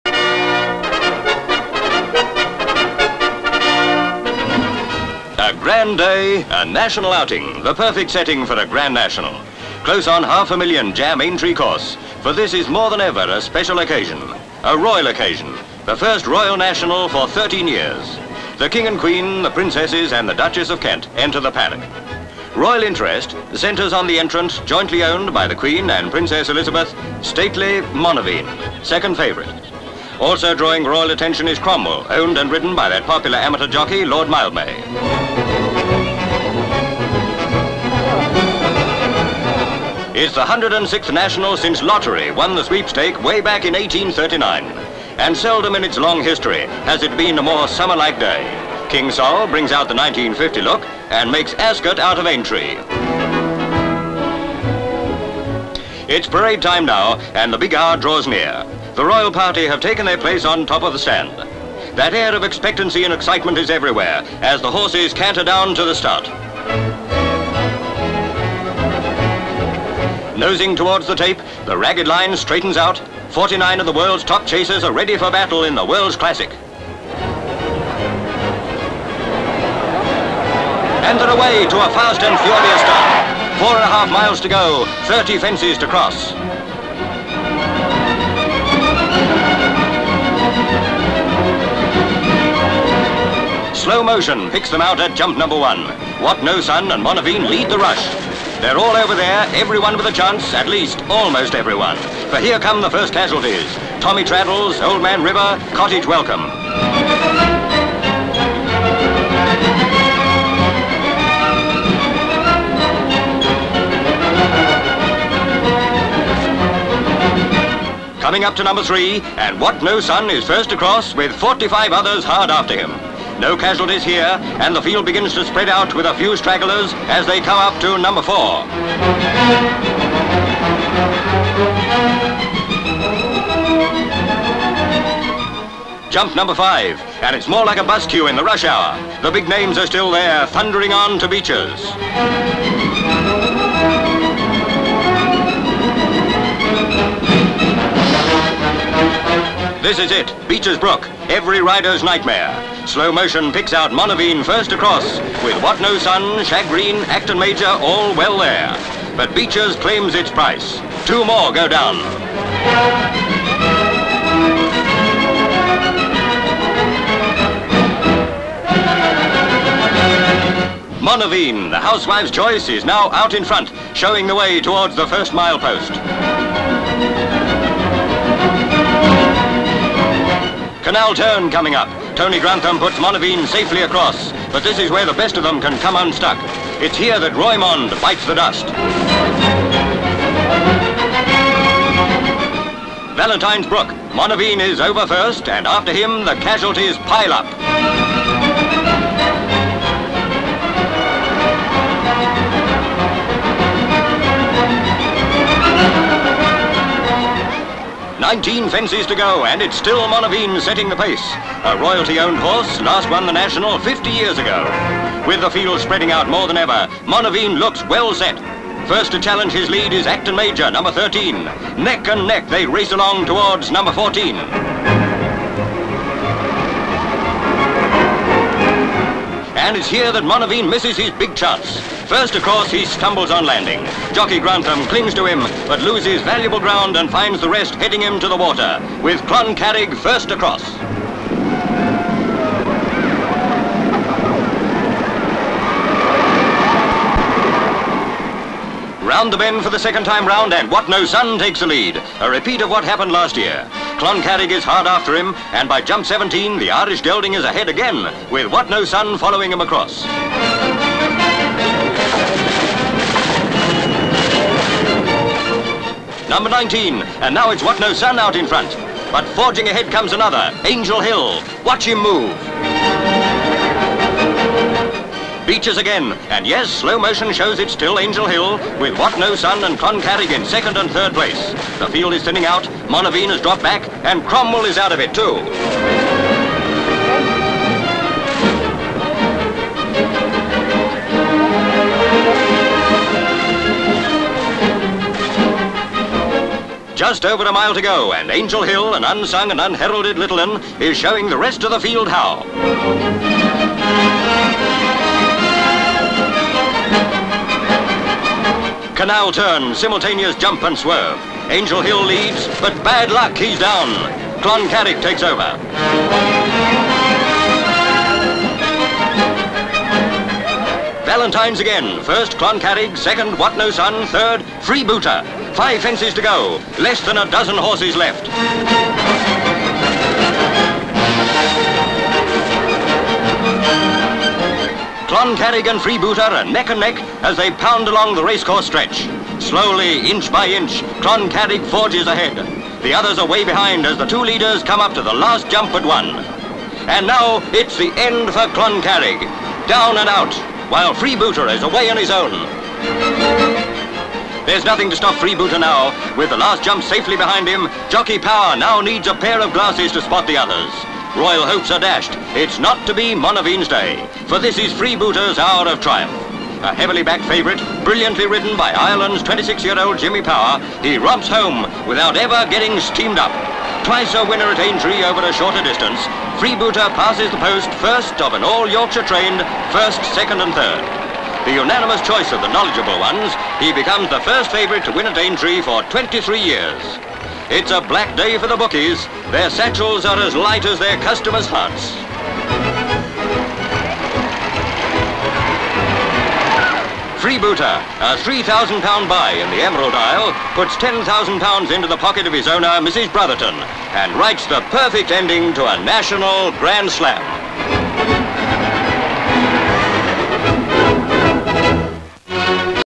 A Royal Grand National (1950)
Full title reads: "A Royal Grand National".

Aintree, Liverpool, Merseyside. Grand National Horse race won by 'Freebooter'.

GV paddock with horses parading. LV Elevated royal party entering paddock. SV horses parading in paddock. SV Queen Elizabeth (later Queen Mother) and Princess Elizabeth (later Queen Elizabeth II) talking to Lord Mildmay. SV Pan 'Monaveen' being led round ring. SV Pan 'Cromwell' being led round ring. CU Queen talking to Mildmay pan to King George VI talking to 'Monaveen's' jockey. VS Jockeys mounting. GV Crowd in front of stand. GV People queuing up at Tote. VS of women wearing fashionable hats. GV Elevated parade. SV Pan Royal Party looking at parade from box. LV horses cantering down to start. SV Crowd. LV Horses lined up at start. SV Crowd looking through binoculars. LV Side view horses lined up at start. Pan as horses start. MV Pan to back view horses racing up to first jump. SV King and Queen looking on. VS of horses racing. CU Woman looking through glasses. MV Pan horses approaching Beechers Brook. Slow motion as they jump. SV Back view from hedge of horses jumping. Two horses fall. MV Pan field led by 'Monaveen' approaching jump No 7. VS of more of the race - 'Monaveen' still leading. Pan between jumps 12 and 13. 'Monaveen' in lead challenged by 'Achton Major'. VS More shots of race horses featured include 'Monaveen', 'Cloncarrig' and 'Wot No Sun', 'Angel Hill' and 'Freebooter'. LV Towards 'Cloncarrig' and 'Freebooter' well ahead of rest of field approach jump 29. 'Cloncarrig' over but falls pan as 'Freebooter' races ahead. LV 'Freebooter' well ahead of field taking last jump. Jockey looks behind. SV Royal Party looking through binoculars. LV 'Freebooter' well ahead of field racing along home straight. LV Head on 'Freebooter' racing along home straight. Passes post pan as he slows down. SV King, Princess Elizabeth and Princess Margaret in Royal Box. SV Queen talking to member of Party. GV 'Freebooter', escorted by police, approaching enclosure. SV Woman owner Mrs Brotherton with 'Freebooter'. Jimmy Power, dismounts and pats horse.

(Lav.) Selected Originals exist for this item - see other records.
 FILM ID:1460.07